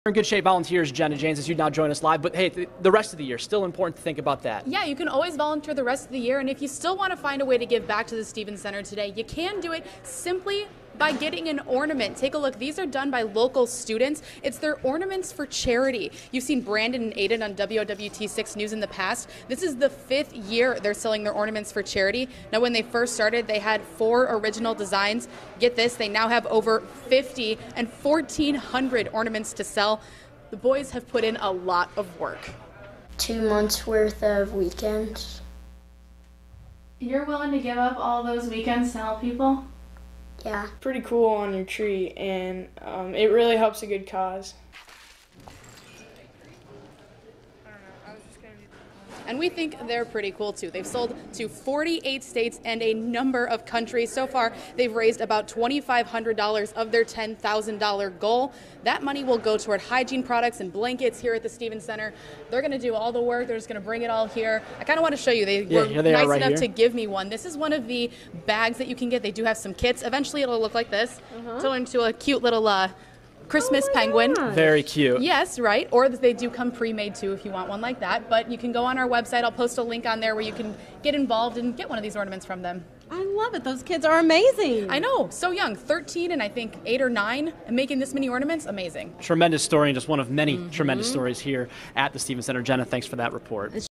Ornaments 4 Charity
How one local family is helping the Stephen Center with ornaments.